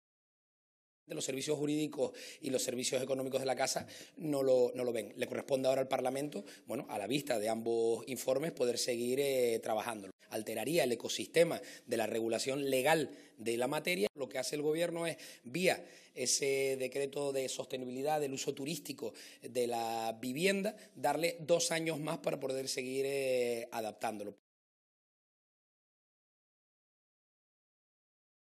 El Gobierno no se opondrá a la iniciativa en el Parlamento de los afectados de la Ley Turística
El Gobierno de Canarias ha asegurado que no se va a situar en contra de la iniciativa popular del turismo en Canarias presentada en el Parlamento de Canarias con casi 23.000 firmas por la Asociación de Afectados por la Ley Turística.

Declaraciones: Alfonso Cabello, portavoz del Gobierno de Canarias.

Informa: Redacción Televisión Canaria.